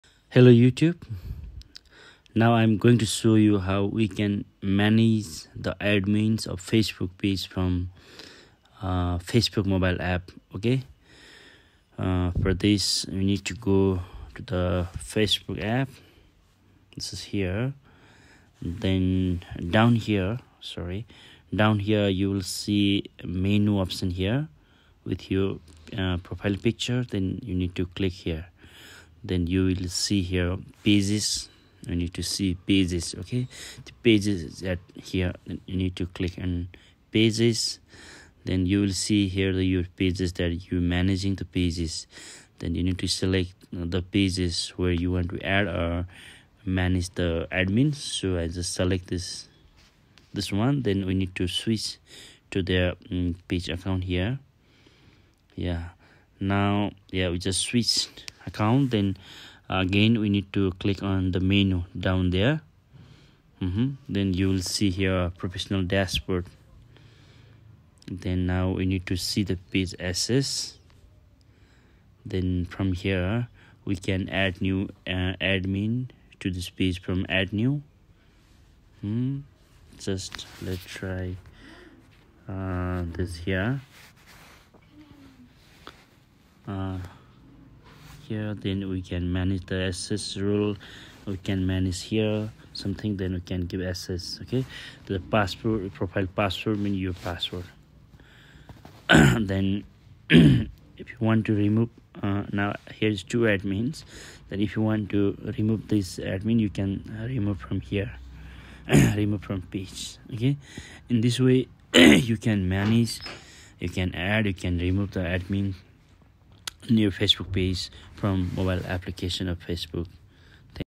How to add new admin to Facebook page from mobile app | facebook page admins management 2024
Effortless Guide: Adding a New Admin to Your Facebook Page Using the Mobile App

Welcome to our step-by-step tutorial on how to easily add a new admin to your Facebook Page directly from your mobile device! If you're managing a business page having multiple admins can streamline your efforts and ensure smooth operation.

In this comprehensive guide, we'll walk you through the entire process, from accessing your Facebook Page on the mobile app to granting admin privileges to another user. No need to switch to a desktop computer or struggle with complex settings – we've got you covered with simple, straightforward instructions.

- Open Facebook Mobile app
- find the pages
-find professional dashboard
-select the page
-switch profile
-find page access
-manage admins